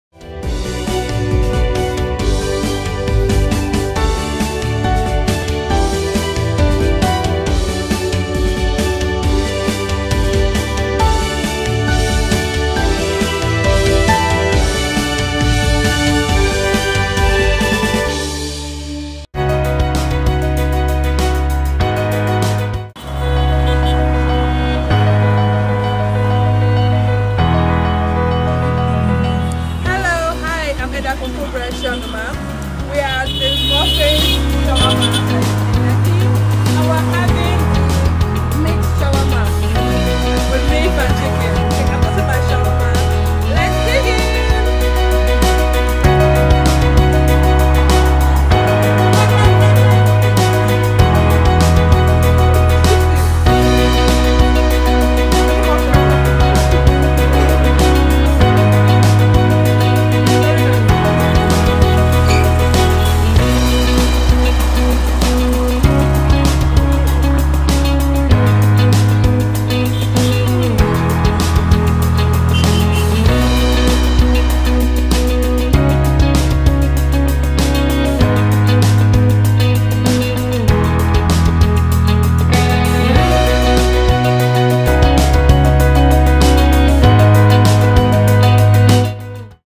Let's Dig In ( Murphis Shawarma lekki)
Welcome Back to this Channel.
I had the mixed Shawarma from Murphis Shawarma lekki. It was very tasty and juicy. You should try it.